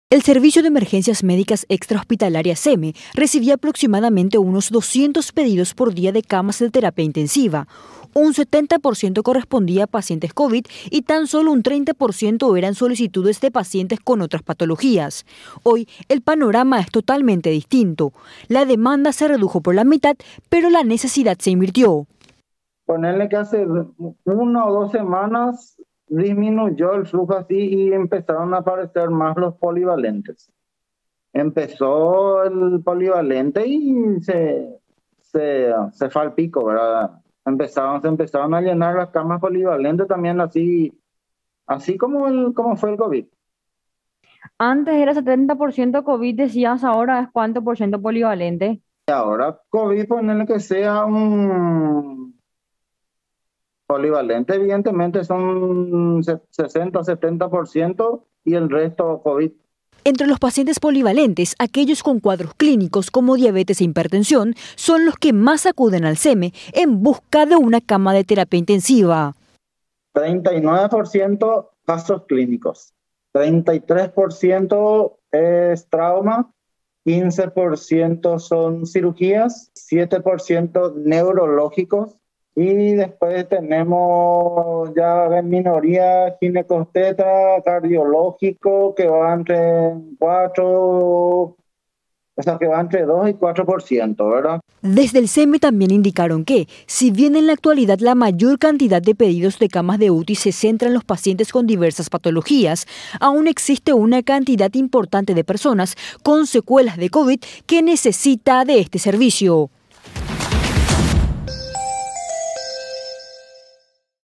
Pedidos de camas de UTI por covid se redujeron, pero aumenta cantidad de pacientes polivalentes
Los pedidos de camas de terapia intensiva disminuyeron un 50% en las últimas semanas. No obstante, la demanda de pacientes polivalentes aumentó de forma drástica, alcanzando los números que anteriormente correspondían a pacientes COVID.